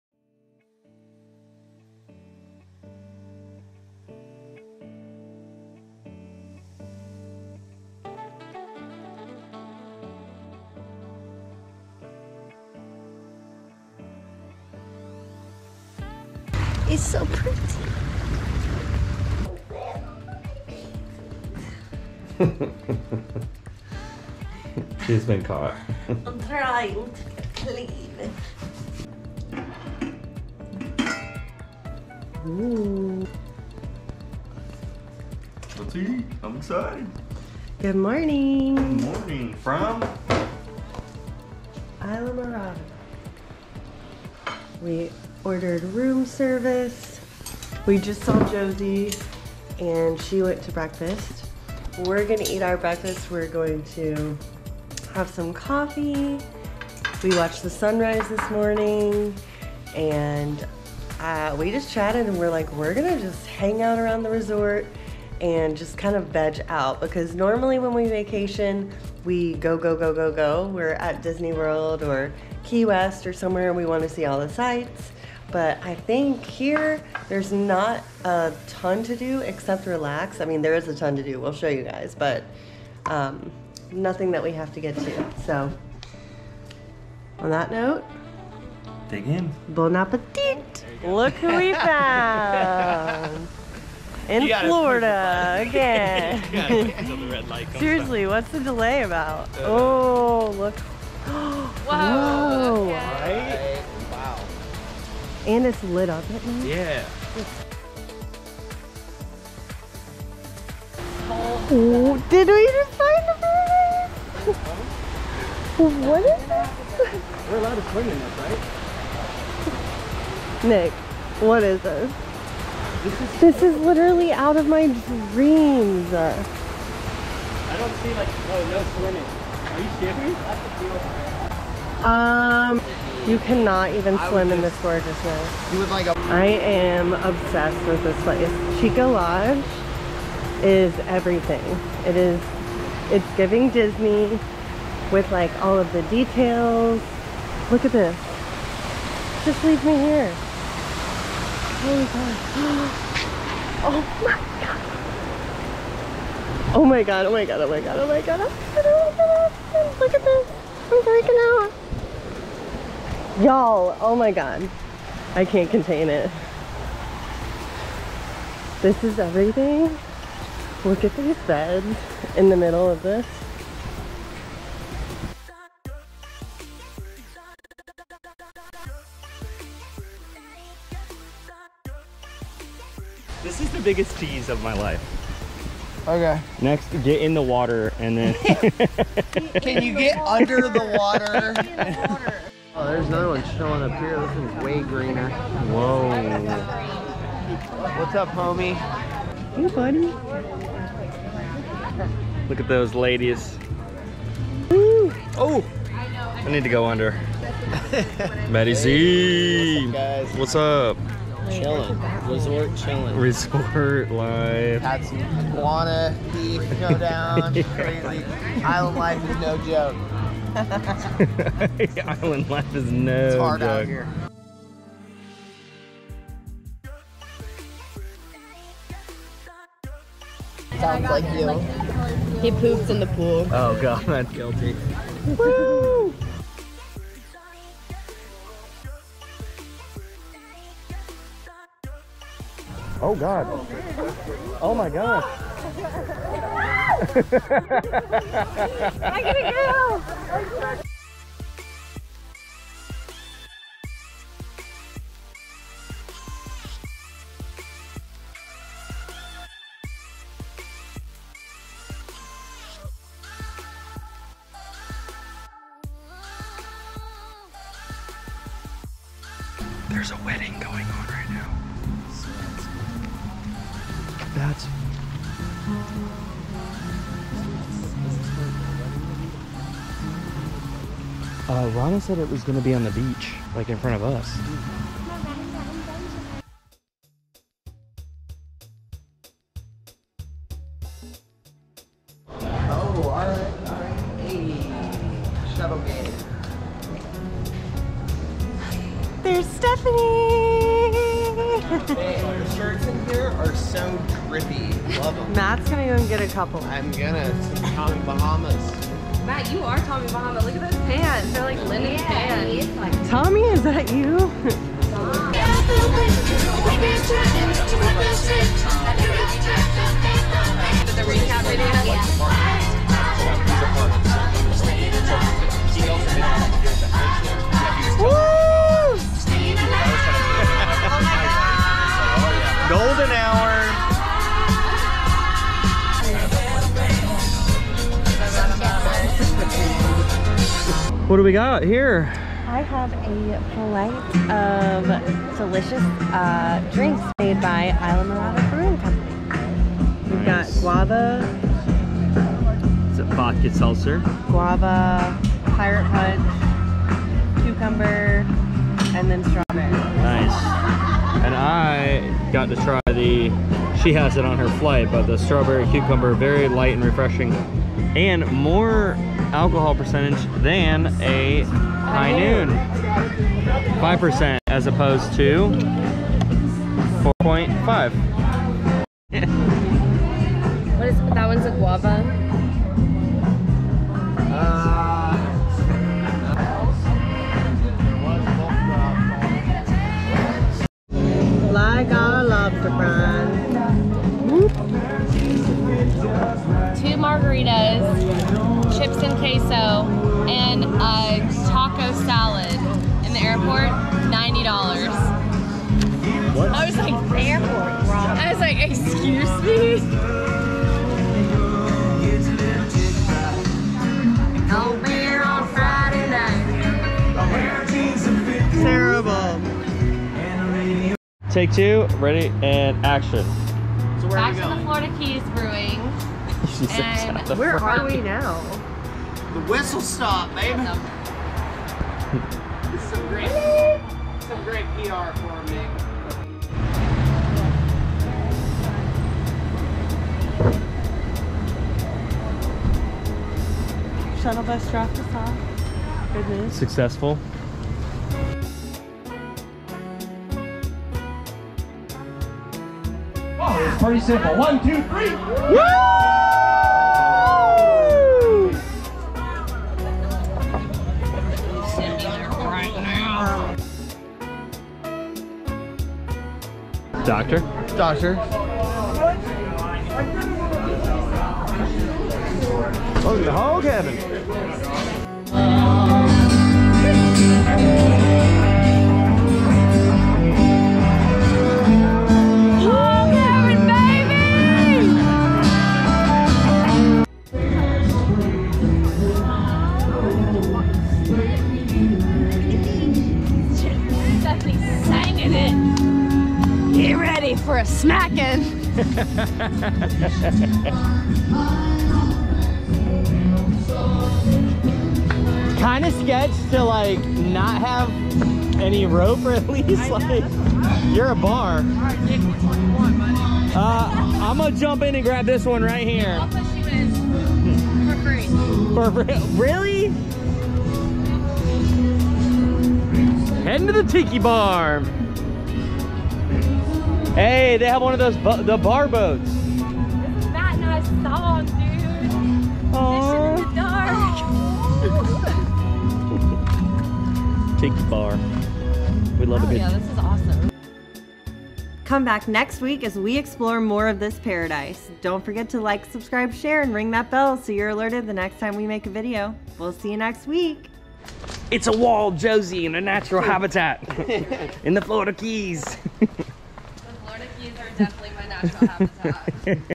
Look Who We Found in Florida! Exploring Islamorada With Our People! Beachfront at the Cheeca Lodge!
Explore Islamorada with us! We settle into Paradise (Cheeca Lodge) as we get ready to celebrate a beautiful wedding.